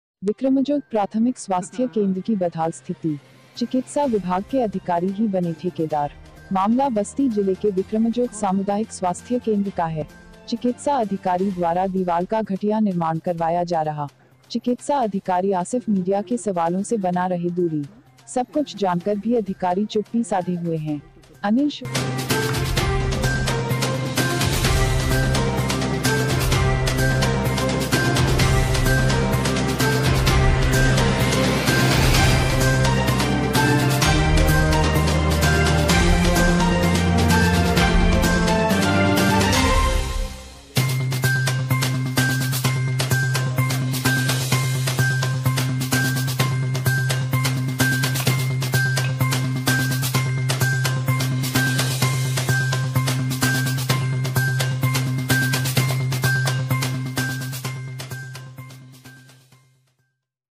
अधिकारी अपनी कंपनी खोलकर कर रहे हैं घन उगाही देखिये पूरा माजरा विडियो में
एंकर।

यू पी योगी सरकार  जहां भ्रष्टाचार पर लगाम लगाना चाहती है  वहीं पर अधिकारियों द्वारा  स्वयं ठेका का रजिस्ट्रेशन करा कर के  विभाग को लूटने का काम कर रहे हैं एक ऐसा मामला बस्ती जिले के विक्रमजोत सीएससी पर चिकित्सा अधिकारी द्वारा घटिया दीवाल निर्माण किया जा रहा है एवं पूछने पर बताते हैं ठेकेदार द्वारा कार्य चल रहा है

बाउंड्री वाल का निर्माण हो रहा है घटिया ईटों से निर्माण किया जा रहा है पूछने पर कोई जवाब देने  से कतराते हैं चिकित्सा अधिकारी आसिफ जी कैमरे पर बोलना नहीं चाहते हैं वहीं चिकित्सा अधिकारी के कहने पर गोलमोल जवाब देते हुए बड़े बाबू ने कहा अस्पताल में बजट आया था इसलिए उसका निर्माण स्वयं करा रहे हैं जबकि ठेकेदार द्वारा कार्य होना चाहिए था

यहां मरीजों को शोषित भी किया जाता है उनसे जबरन पैसा वसूला जाता है डिलीवरी के दौरान यह नमो डॉक्टरों द्वारा अच्छी रकम ली जाती है विक्रमजोत सीएससी में भ्रष्टाचार चरम पर है लेकिन प्रदेश के अधिकारी कुंभकरण की नींद में सो रहे हैं